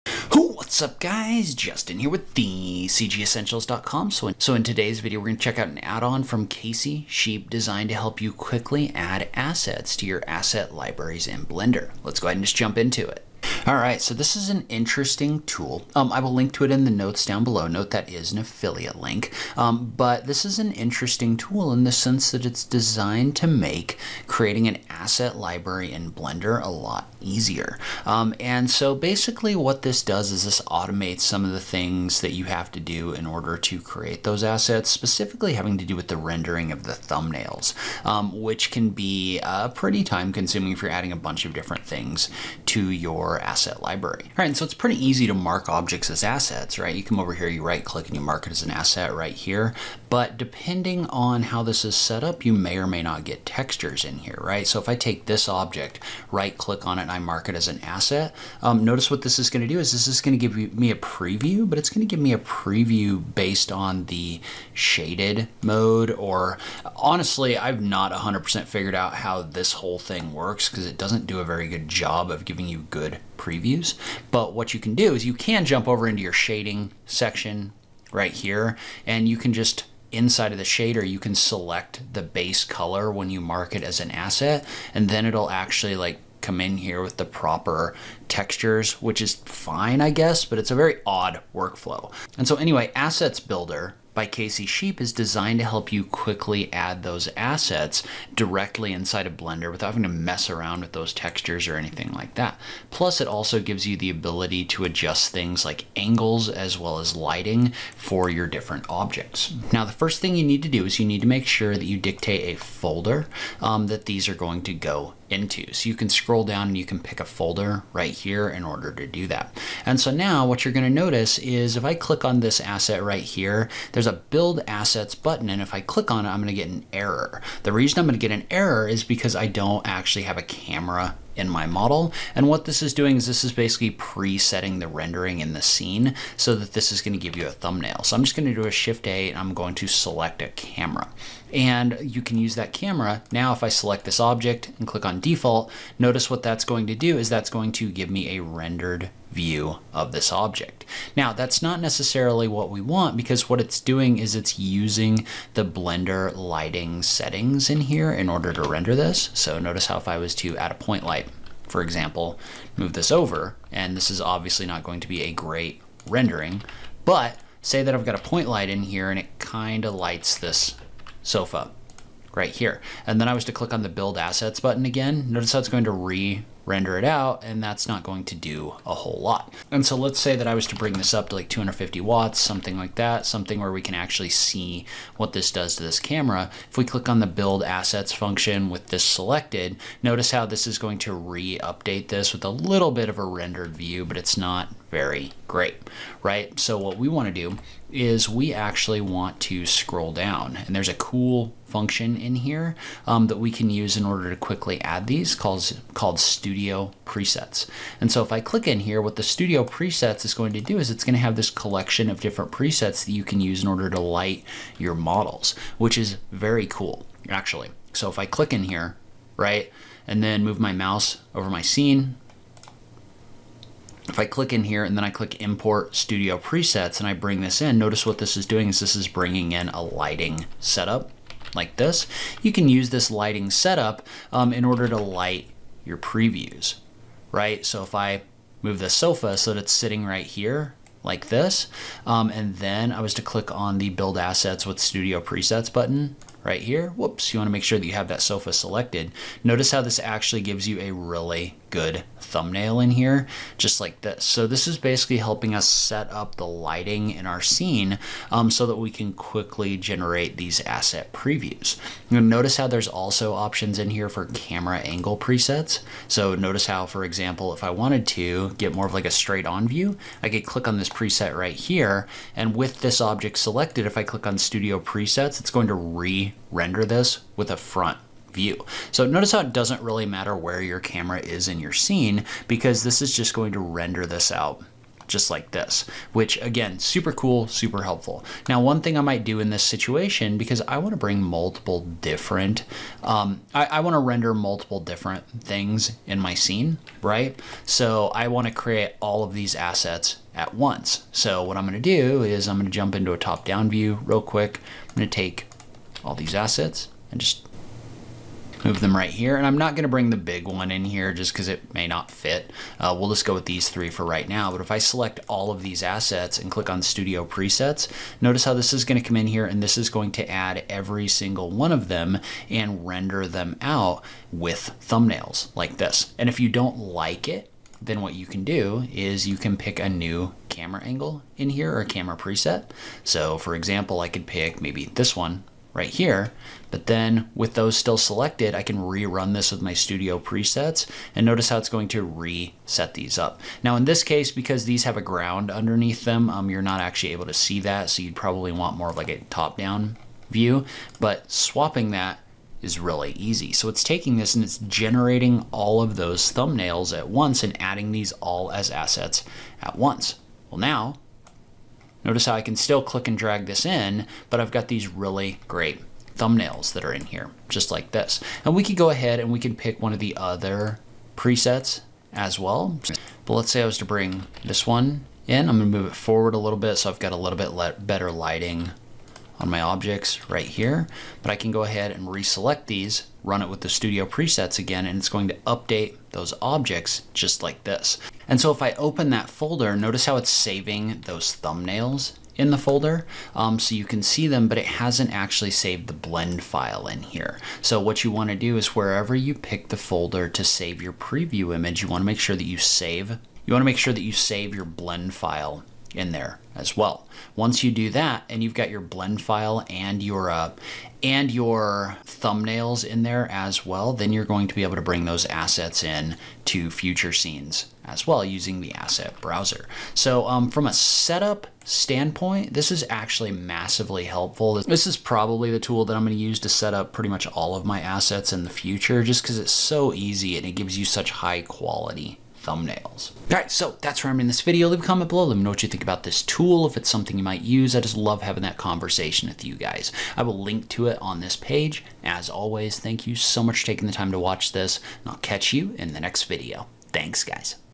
The EASIEST Way to Build Asset Libraries in Blender is HERE!
In this video, we check out a tool from Casey Sheep designed to help you quickly create asset libraries with awesome looking thumbnails for Blender! You COULD also use the lighting setups in your models for renderings as well :)

TIMESTAMPS
0:00 - Introduction
0:11 - Where to find it
0:52 - Typical asset creation process in Blender
1:40 - Using Assets Library Builder to create assets and thumbnails
3:33 - Using the asset lighting setups to create rendered asset thumbnails
6:13 - Changing thumbnails and saving assets for future use